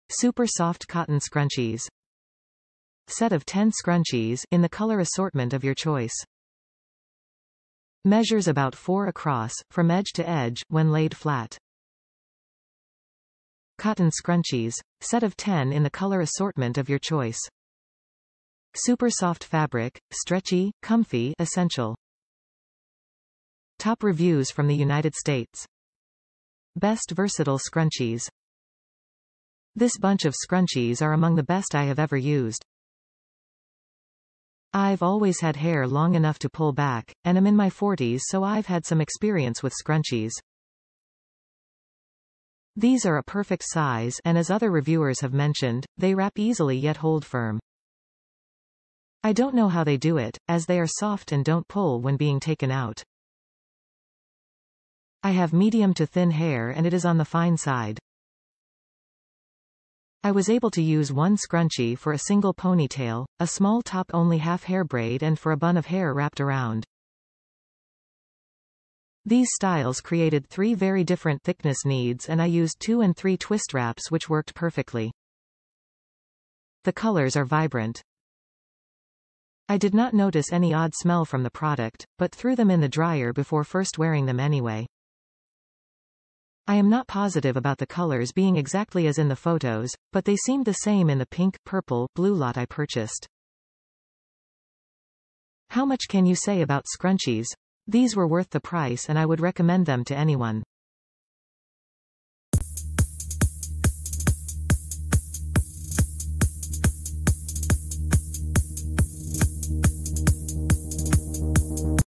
Cotton Scrunchie Set, Set of 10 Soft Cotton Scrunchies (Bright Color Assortment)
Super soft cotton scrunchies
Set of 10 scrunchies, in the color assortment of your choice
Measures about 4" across, from edge to edge, when laid flat

Cotton scrunchies! Set of 10 in the color assortment of your choice. Super soft fabric - stretchy, comfy, essential!

Best Versatile Scrunchies

This bunch of scrunchies are among the best I have ever used. I've always had hair long enough to pull back, and am in my 40's so I've had some experience with scrunchies. These are a perfect size, and as other reviewers have mentioned, they wrap easily yet hold firm. I don't know how they do it, as they are soft and don't pull when being TAKEN out. I have medium to thin hair and it is on the fine side. I was able to use one scrunchie for a single pony tail, a small top only half hair braid and for a bun of hair wrapped around. These styles created three very different thickness needs and I used two and three twist wraps which worked perfectly.
The colors are vibrant. I did not notice any odd smell from the product, but threw them in the dryer before first wearing them anyway.
I am not positive about the colors being exactly as in the photos, but they seemed the same in the pink/purple/blue lot I purchased.
How much can you say about scrunchies? These were worth the price and I would recommend them to anyone.